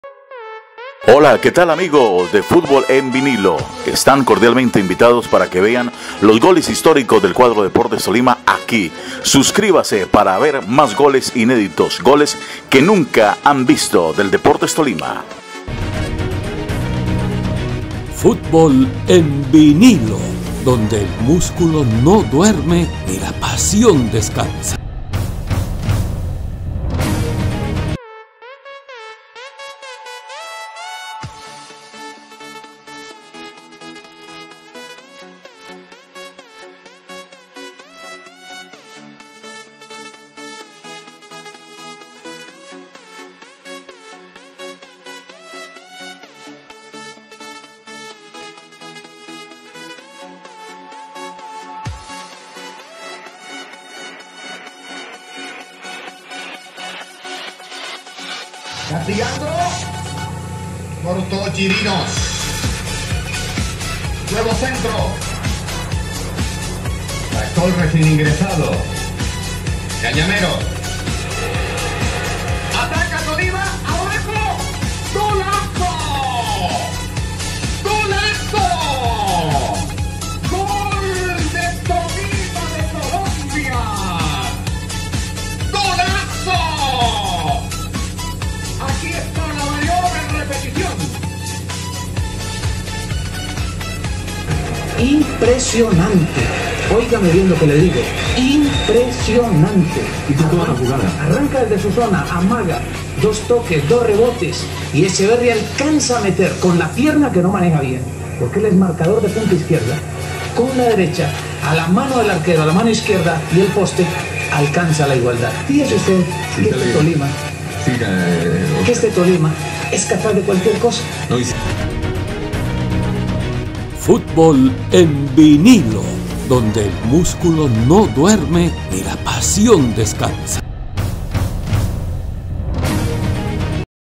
COPA LIBERTADORES 1983 UNIVERSITARIO DE DEPORTES 2 VS DEPORTES TOLIMA 2 FUTBOL EN VINILO TV
LOS MEJORES GOLES DE LA HISTORIA DEL DEPORTES TOLIMA LO HIZO EL RATON ECHEVERRY  GOOOOOOOOOOOL MARADONIANO,,,,DISFRUTELO,